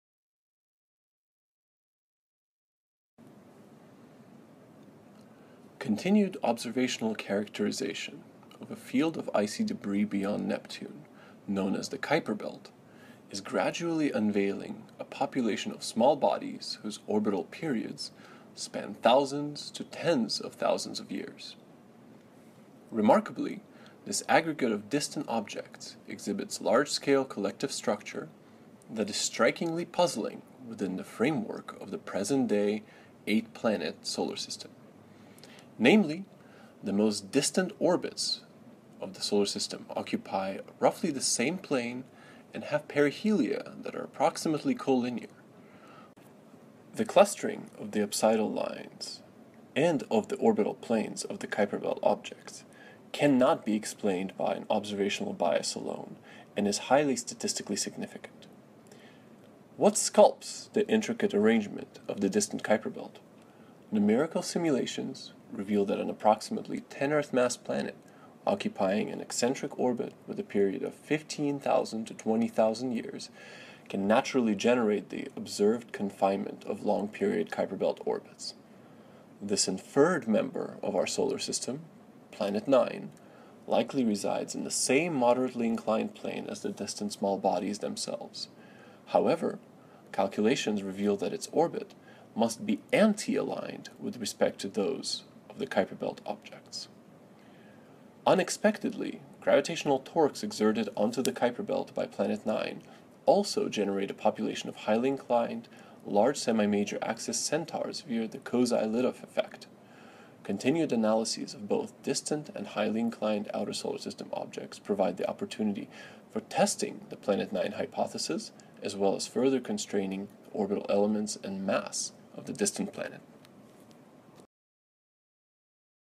Video Abstract Planet9
EVIDENCE FOR A DISTANT GIANT PLANET IN THE SOLAR SYSTEM
Konstantin Batygin and Michael E. Brown
Astronomical Journal 151, page 22, 2016 February

Continued observational characterization of a field of icy debris beyond Neptune, known as the Kuiper belt, is gradually unveiling a population of small bodies, whose orbital periods span thousands to thens of thousands of years. Remarkably, this aggregate of distant objects exhibits large-scale collective structure, that is strikingly puzzling within the framework of the present-day 8-planet solar system. Namely, the most distant orbits of the solar system occupy roughly the same plane, and have perihelia that are approximately co-linear. The clustering of the apsidal lines and of the orbital planes of the Kuiper belt objects cannot be explained by an observational bias and is highly statistically significant.

What sculpts the intricate arrangement of the distant Kuiper belt? Numerical simulations reveal that an approximately 10 Earth mass planet, occupying an eccentric orbit with a period of 15,000-20,000 years, can naturally generate the observed confinement of long-period Kuiper belt orbits. This inferred member of our solar system, Planet Nine, likely resides in the same moderately inclined plane as the distant small bodies themselves. However, calculations reveal that its orbit must be anti-aligned with respect to those of the Kuiper belt objects.

Unexpectedly, gravitational torques exerted onto the Kuiper belt by Planet Nine also generate a population of highly inclined, large semi-major axis Centaurs via the Kozai-Lidov effect. Continued analysis of both distant and highly inclined outer solar system objects provides the opportunity for testing the Planet Nine hypothesis, as well as further constraining the orbital elements and mass of the distant planet.